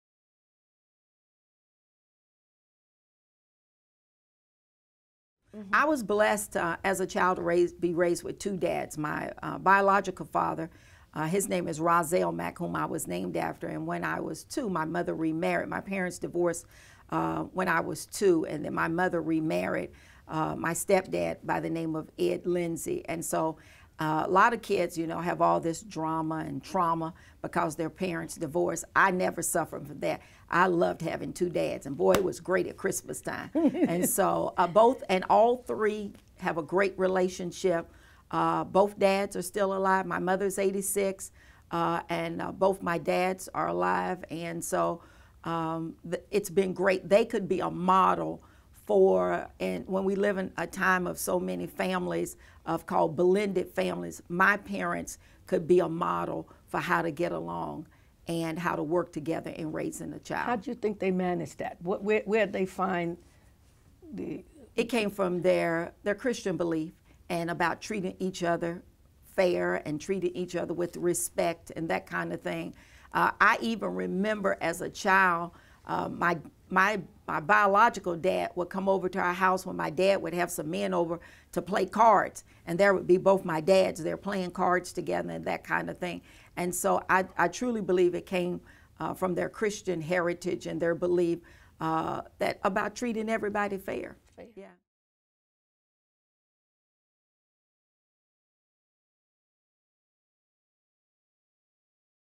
3 Akins: Divorced Family
Akins tells of her family growing up. Her parents got divorced when she was two years old and she has a step-father.

Roszalyn Akins is the First Lady of the First Baptist Church of Bracktown. She started the Carter G. Woodson Academy for young men and taught for 30+ years at Leestown Middle School in Lexington, KY.

This interview took place on November 9, 2015 as part of the Kentucky Civil Rights Hall of Fame Oral History Project, Louie B. Nunn Center for Oral History and University of Kentucky Libraries.

Interviewer: Betty Baye
Videographer: Joanna Hay